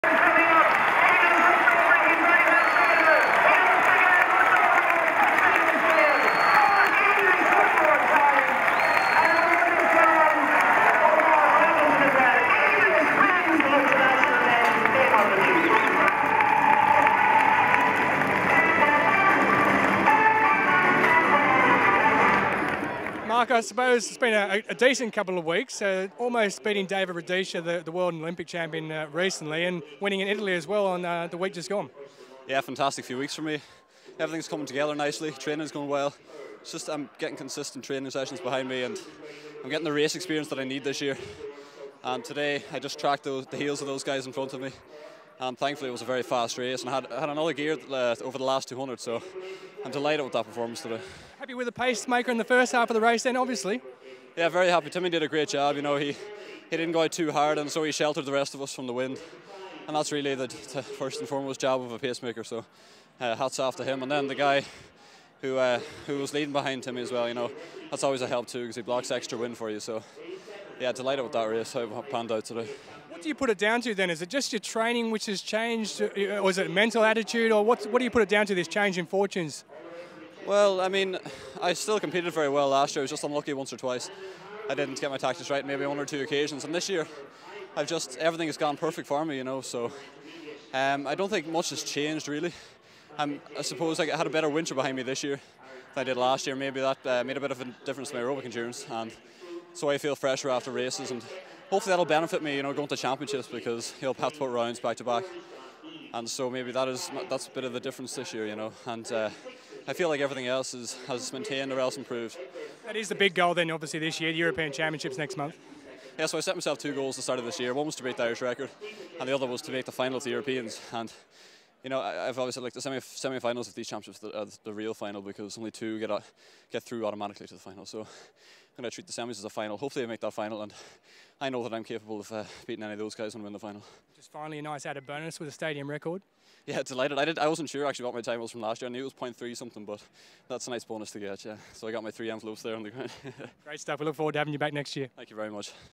Morton Games 2014 - Mark English 800m record
Mark English sets a new stadium record of 1:45.30 in the 800m at the 2014 Morton Games. Finish-line camera shot and post-race interview. Highlights on Setanta Ireland Monday 14 July at 6pm.